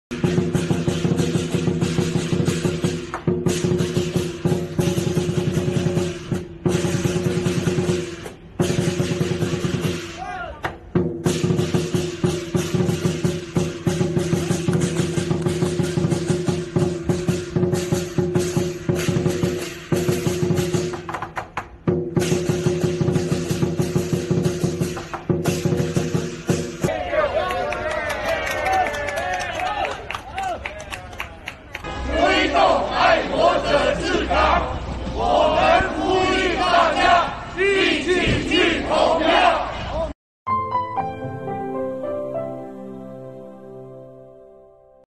香港多个同乡社团舞狮呼吁选民踊跃投票
12月19日，香港第七届立法会选举投票日。福建社团联会、海南社团总会、山东社团总会等多个同乡社团今日在北角、九龙城、观塘、屯门等地组织舞狮庆祝，呼吁广大选民履行公民责任，为港为己，踊跃参与投票，推进爱国者治港，实践新选制，开创新历史。